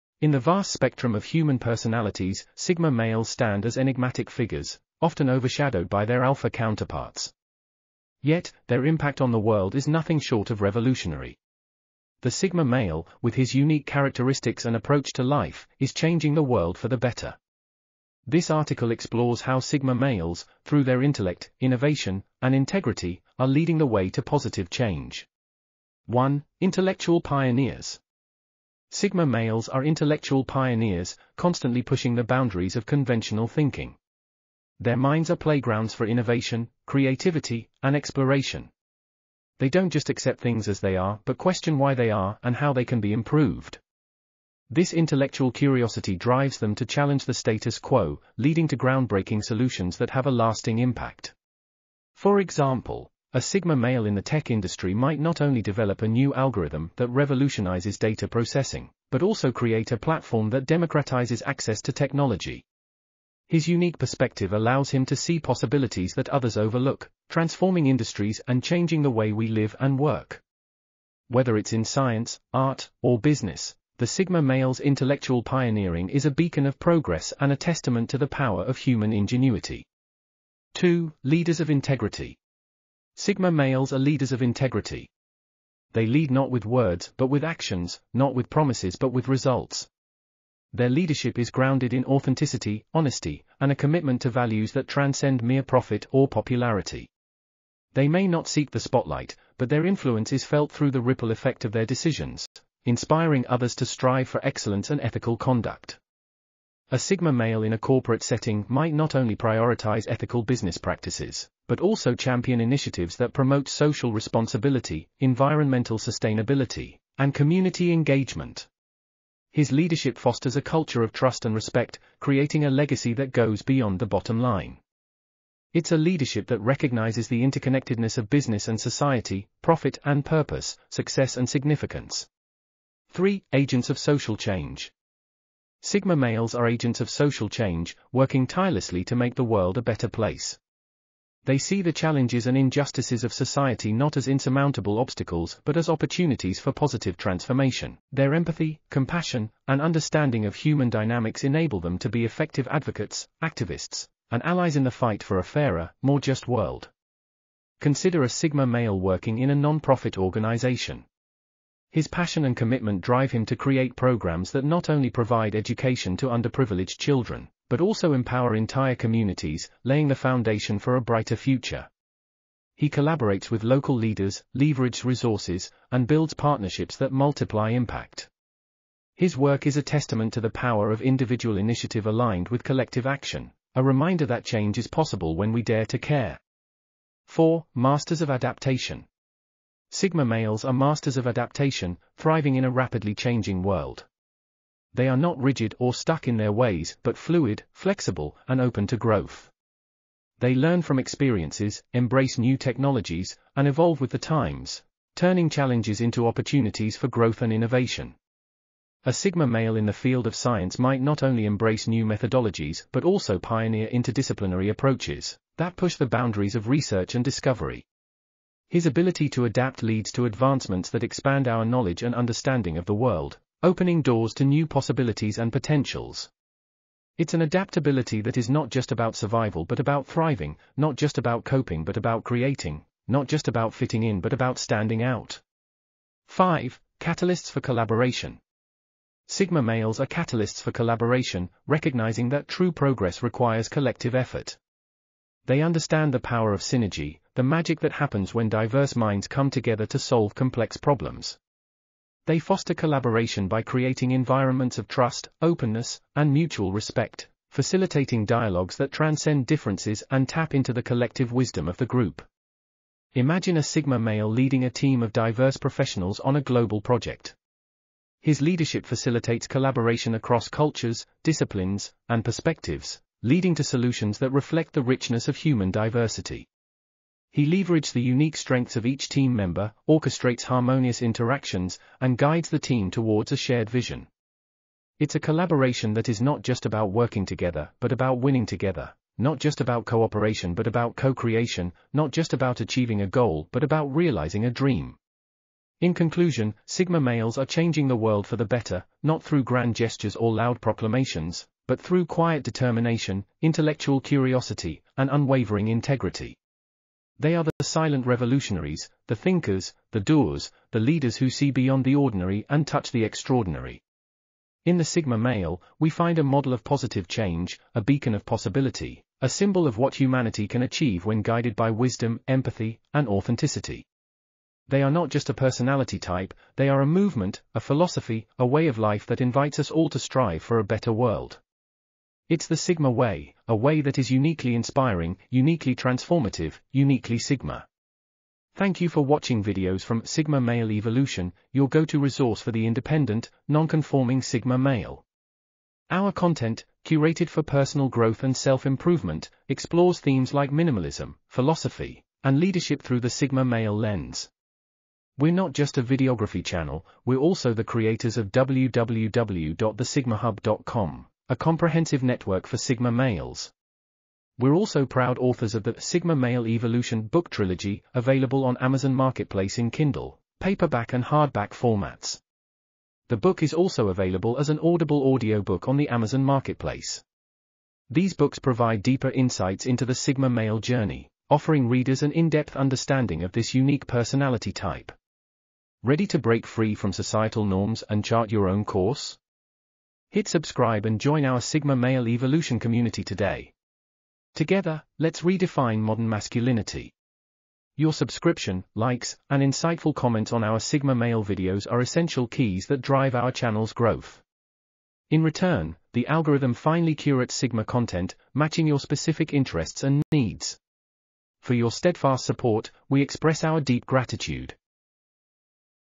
How SIGMA Males Are Changing the World (2024)
Sigma Males: Changing the World One Step at a Time - Uncover Their Secrets!

📚Sigma Male Book Trilogy (Amazon Worldwide):

Join us as we delve into the fascinating world of Sigma males and their profound impact on society. From innovation to leadership, discover how Sigma males are not just followers but trailblazers, leading the way to a better world.